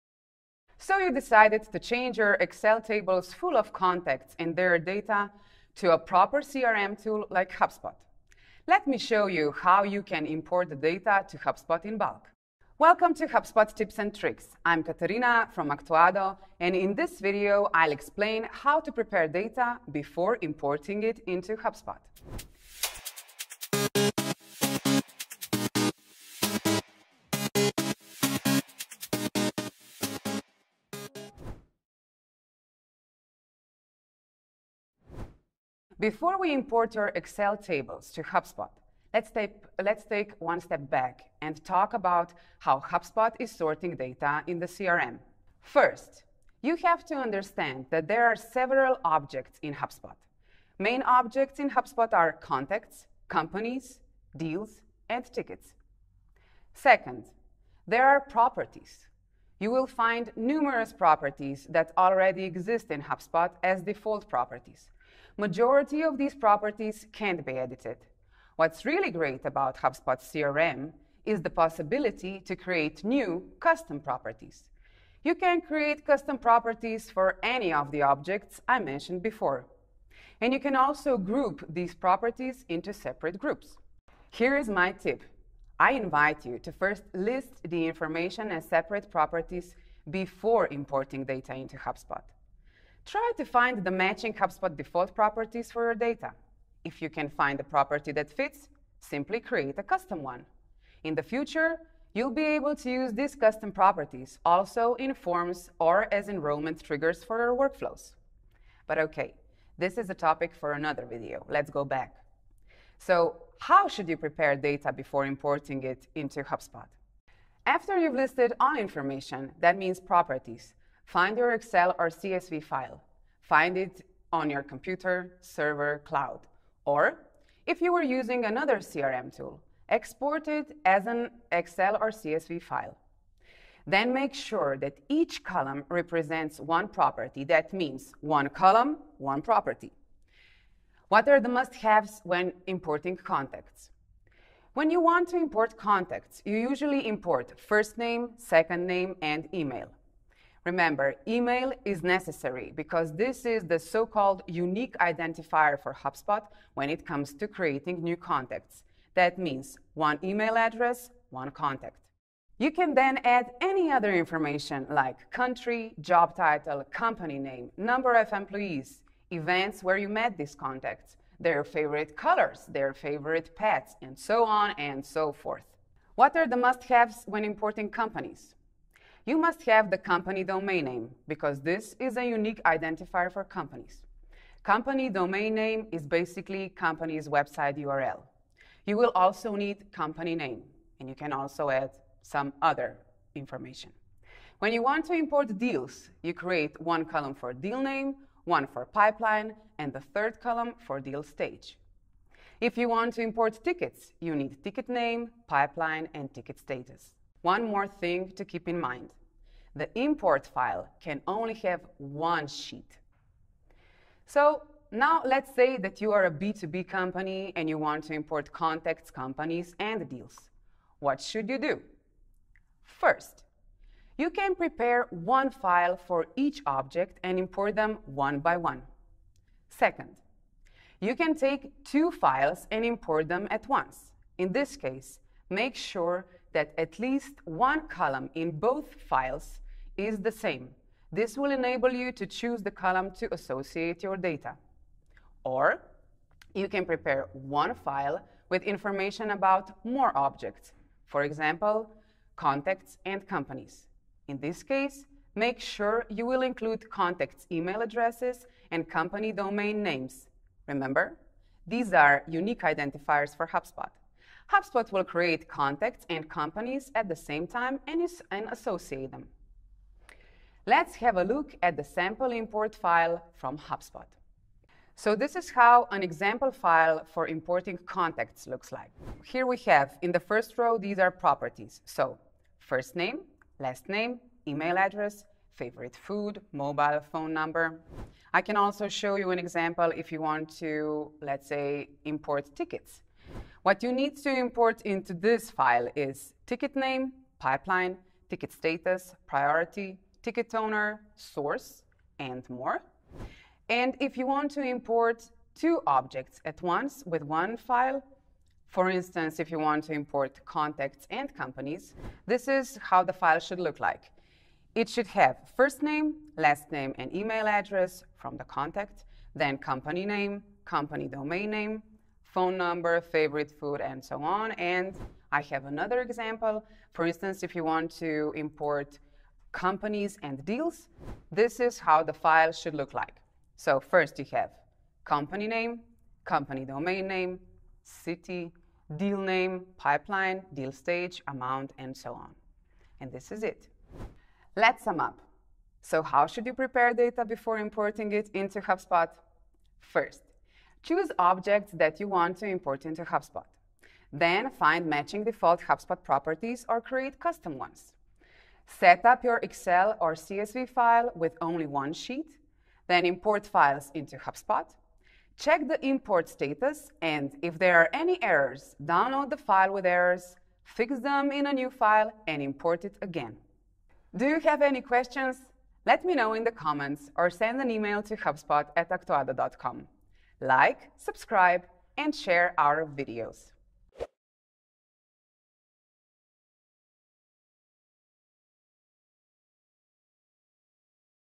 How to prepare data before importing it into HubSpot?
So you decided to change your excel tables full of contacts and their data to a proper CRM tool like HubSpot. Let us show you how you can import the data to HubSpot in bulk.

Do you have any questions?

If you find this video helpful, share it and sign up to be notified when we post a new video!